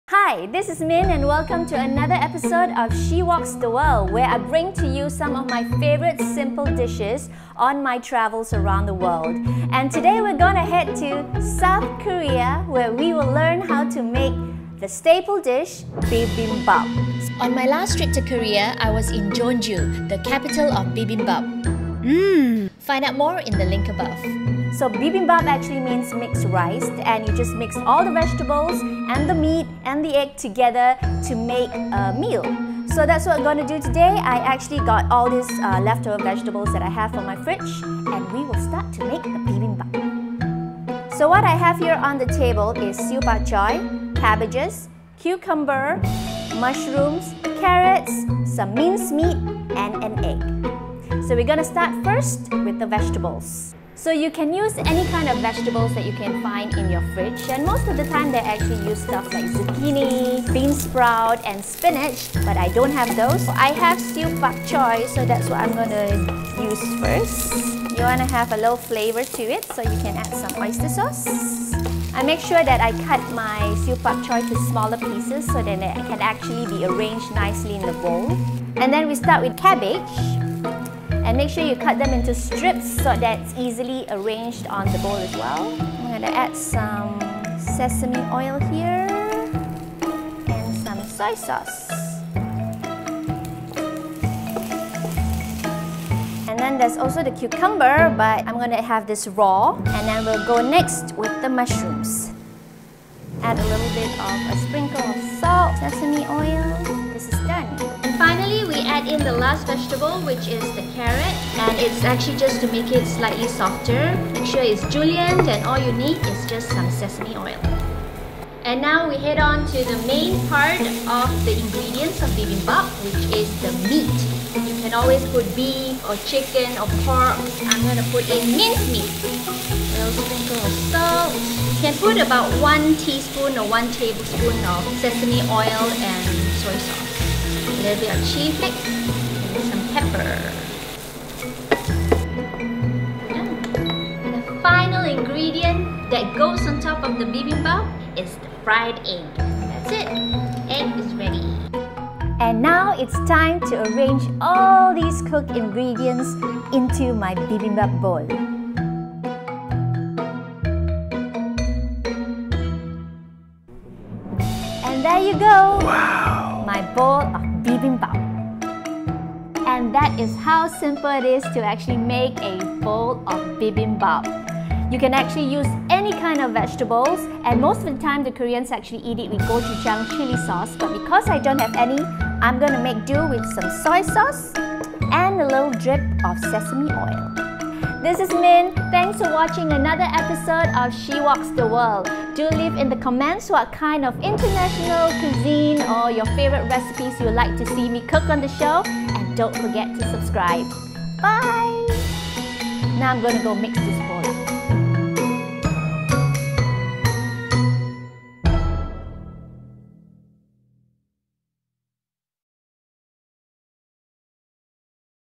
Ep.2 She Woks the World: Bibimbap (South Korea)
She Woks the World -- sharing with you recipes of some of my favourite international dishes that I've tried and tasted on my travels around the world. On this second episode, I tried making a mixed rice recipe from South Korea -- the ever popular Bibimbap! It's a must-eat every time I visit the country.